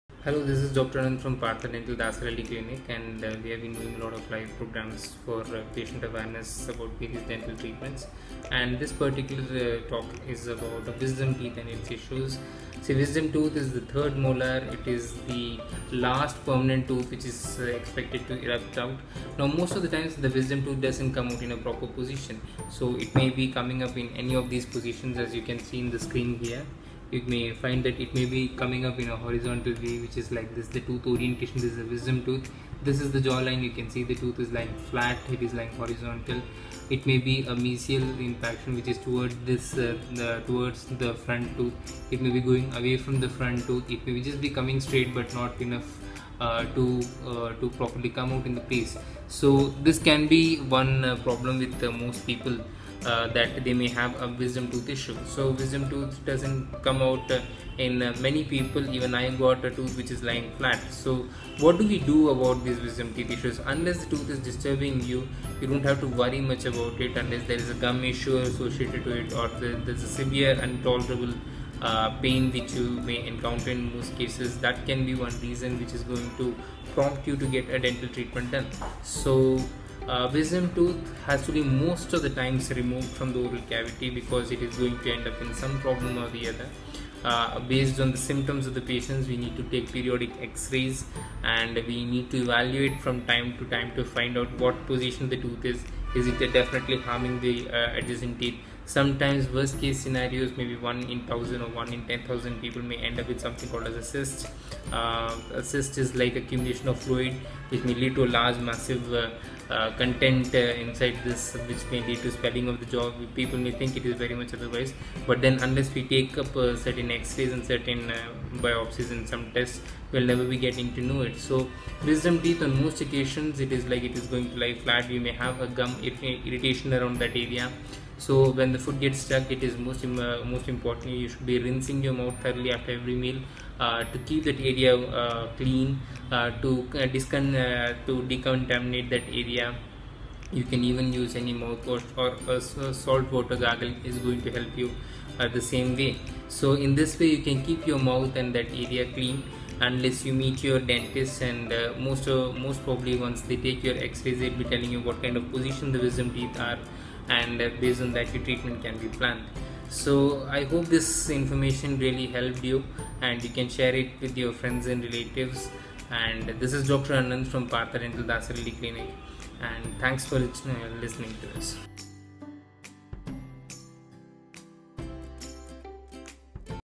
Awareness about wisdom tooth II Partha Dental II
Wisdom tooth is another name for any one of four third molars found in the permanent dentition (adult teeth). These teeth are the last or most posterior teeth in the dental arch. Although most people have wisdom teeth, it is possible for some or all of the third molars .